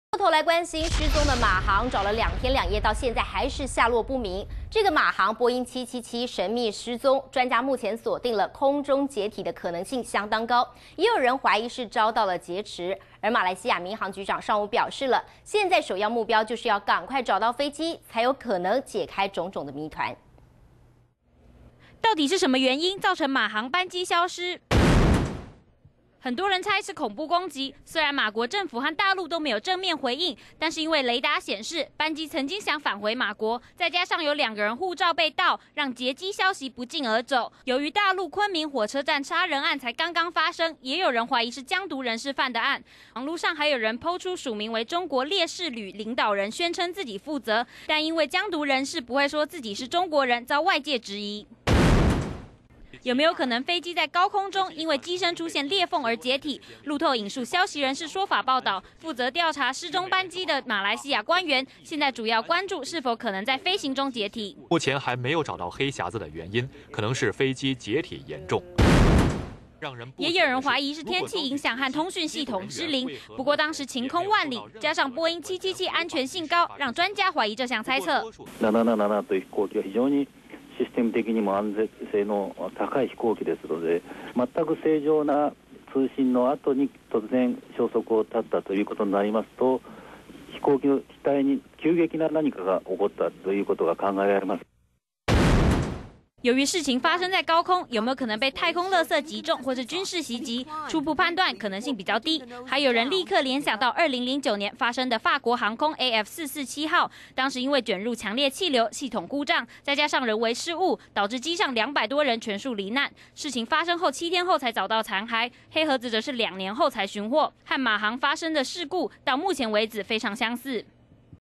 【中視新聞】馬航神祕消失 恐攻?故障? 謠言滿天 20140310
找了兩天兩夜還是不落不明，馬航波音777神秘失蹤，專家目前鎖定，空中解體的可能性相當高，也有人懷疑是遭到劫持，馬來西亞民航局長上午表示，現在的首要目標，就是趕快找到飛機，才有可能解開謎團。